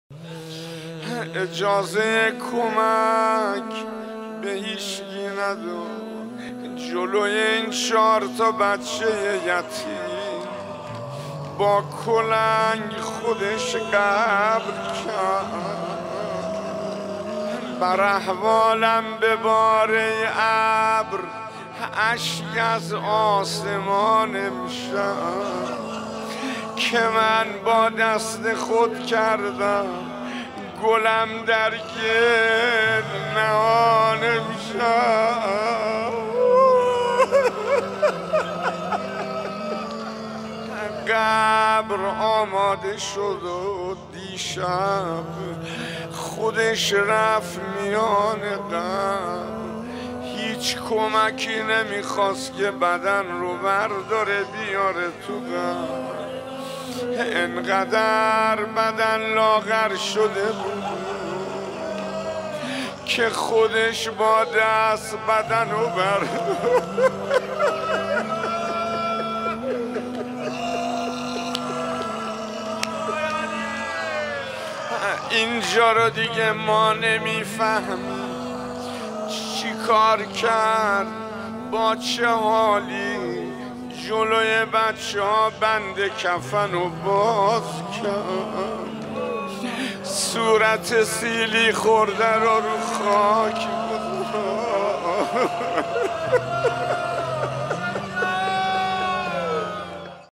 بر احوالم ببار ای ابر، اشک از آسمان امشب... | شیخ حسین انصاریان
اجازه کمک به هیچکس نداد؛ جلوی این چهار تا بچه یتیم با کلنگ خودش قبر کند.

بر احوالم ببار ای ابر اشک از آسمان امشب / که من با دست خود کردم گل اندر گل نهان امشب

قبر آماده شد و خودش میان قبر رفت، هیچ کمکی نمیخواست بدن را بردارد و در قبر بیاورد.

اینقدر بدن لاغر شده بود که خودش با دست بدن را برداشت اینجا را دیگر ما نمی‌فهمیم چکار کرد و با چه حالی جلوی بچه‌ها بند کفن را باز کرد و صورت سیلی خورده را روی خاک گذاشت...

ویژه ایام فاطمیه

️جمادی الثانی ۱۴۰۰ / ویژه‌برنامه روز شهادت
️ تهران / حسینیه هدایت

ما را در اینستاگرام دنبال کنید